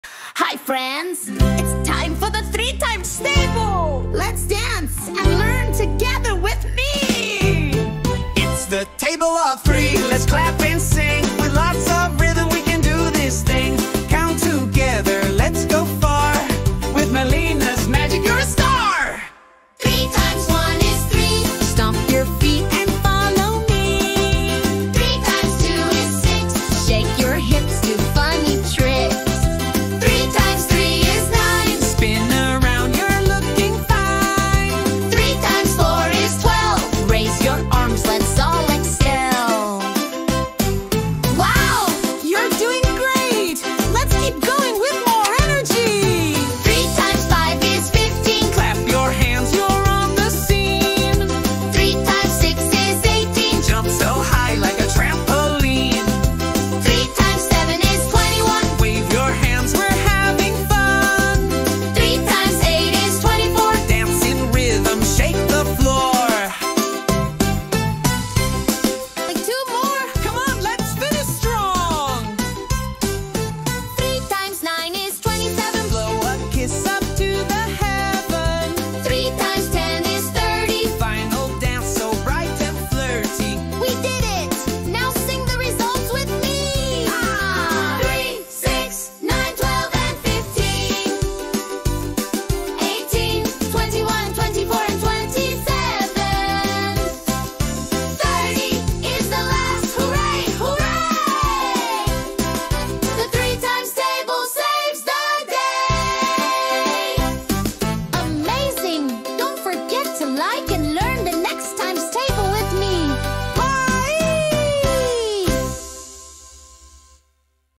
Multiplication Table of 3,
Learn the 3 Times Table with Music and Dance! | Melina Teaches by Singing 🎵

Melina is back with lots of fun and energy! In this short and catchy video, kids will learn the multiplication table of 3 while dancing, singing, and moving their bodies. Perfect for learning math in a joyful and unforgettable way.

👉 Repeat, dance, and sing along to memorize the results!
👉 Ideal for kids between 6 and 12 years old.
👉 Educational + fun = learning made easy!